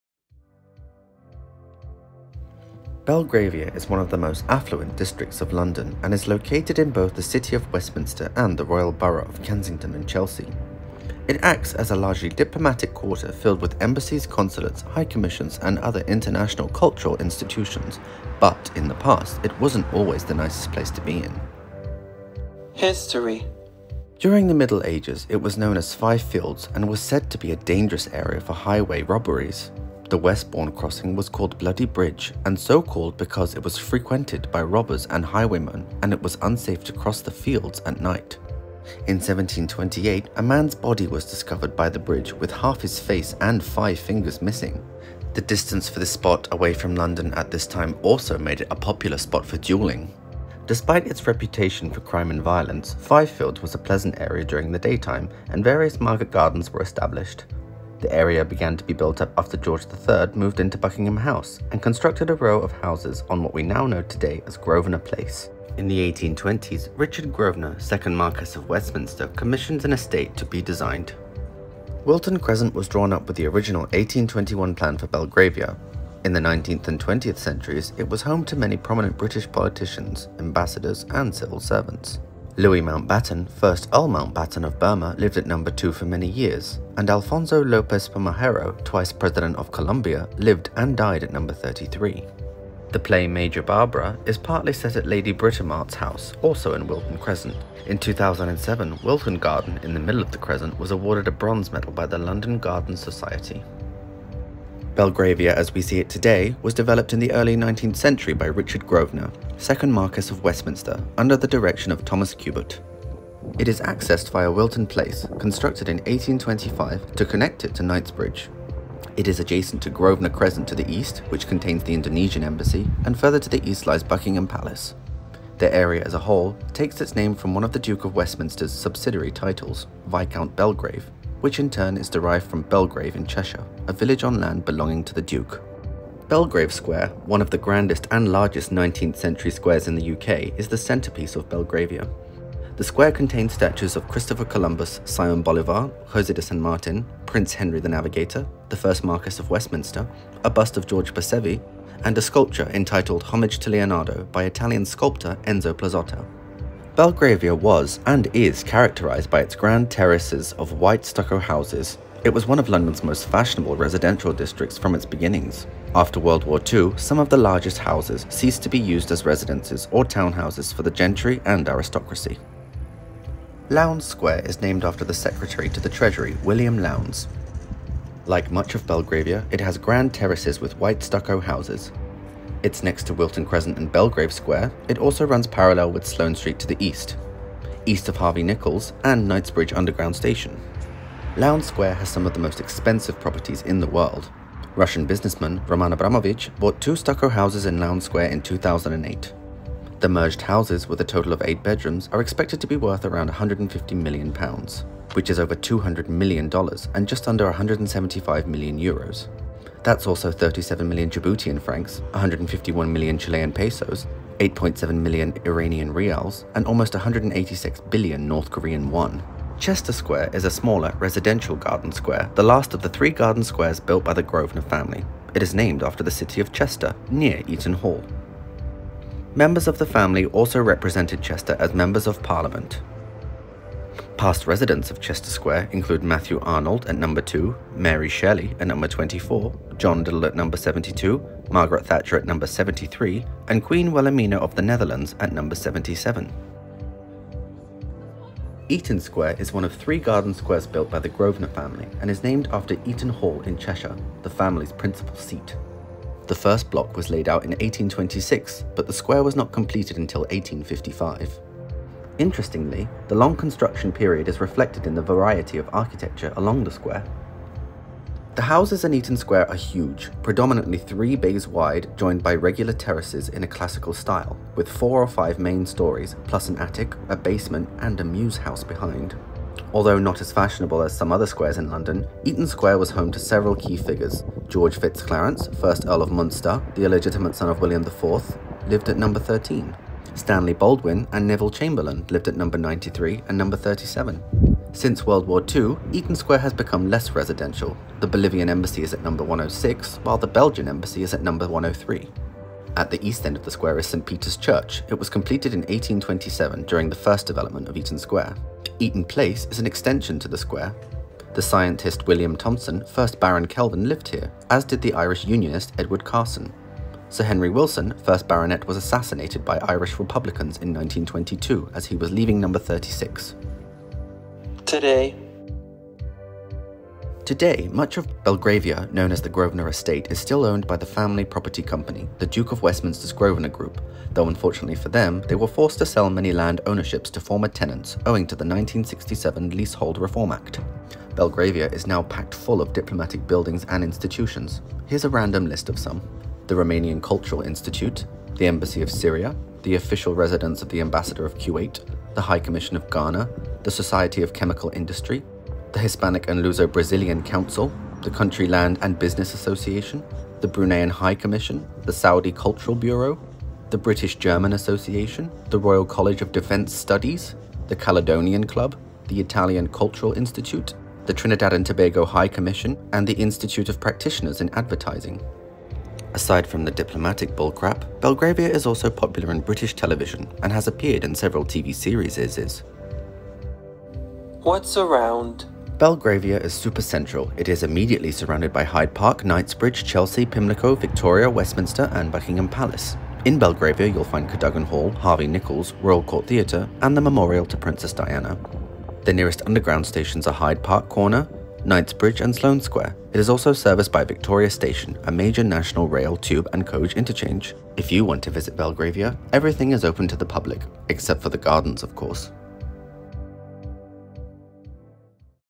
🇬🇧 Belgravia | Rococo diplomatic quarter 🇺🇳
One of the most affluent districts of London, filled with Stucco-style diplomatic buildings from around the world.